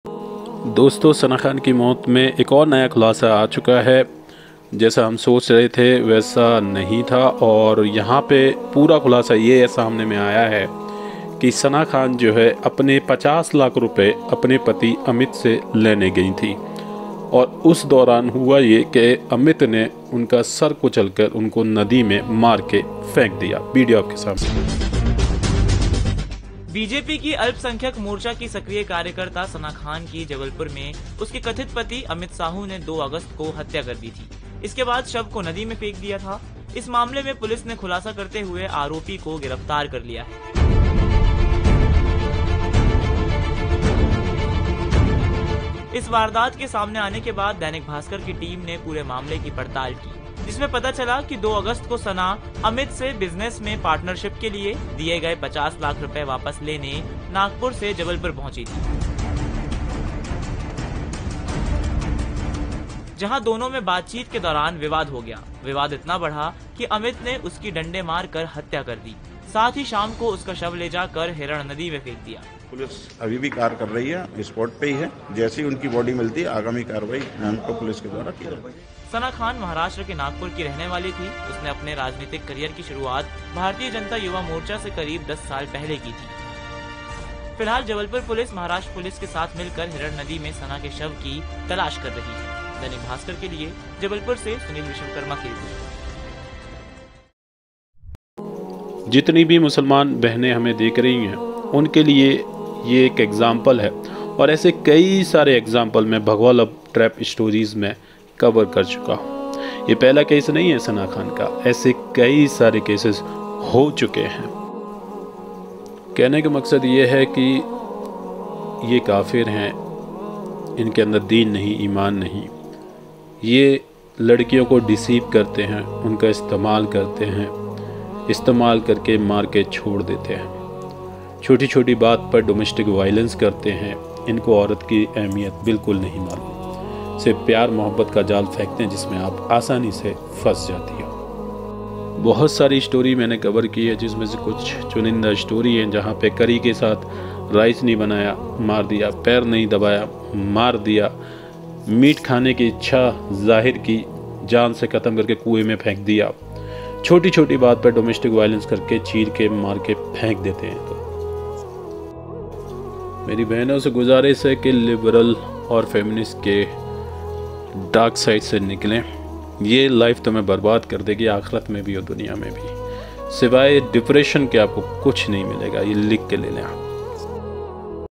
Muslim Sisters Jo Liberalism Aur Feminism Ka Shikar Ho Rahi Hai Unke Liye Sana Khan Ki Maut Sabak H
Etihaad Responds